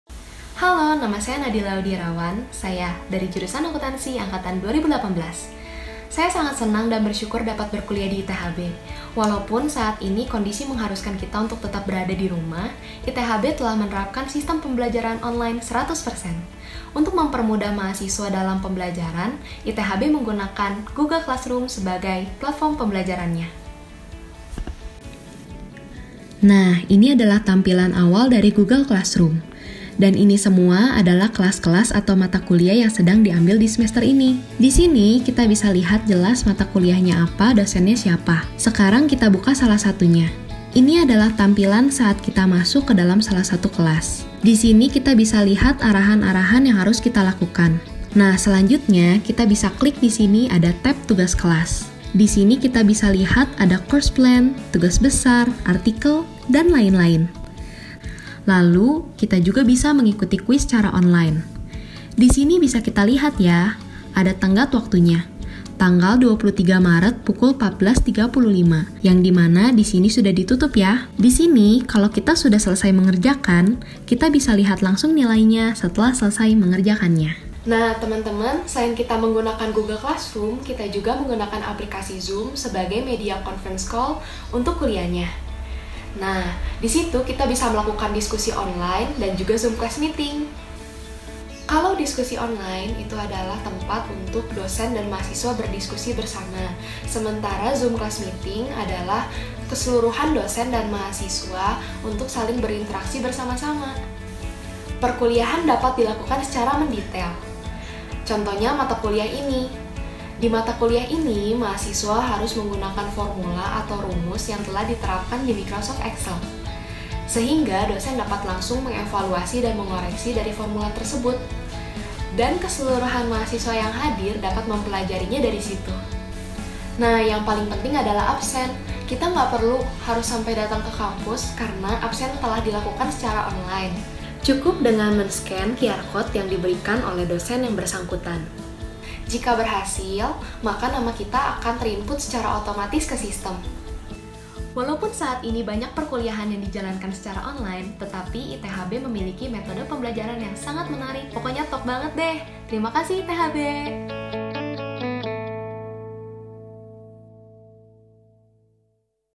Pertama Kali Kuliah Online dengan Kualitas TOP! - Nadilla Audy Irawan
Keadaan dunia tidak ada yang pernah tahu seperti apa. Hingga di saat pandemik virus yang mengharuskan mahasiswa untuk kuliah dari rumah turut mempengaruhi kegiatan belajar mereka.
ITHB pun memberikan jawaban terbaik untuk kekhawatiran tersebut.

Dan ini adalah salah satu mahasiswa ITHB yang telah mengikuti kuliah online di ITHB, Nadilla Audy Irawan dari Akuntansi 2018.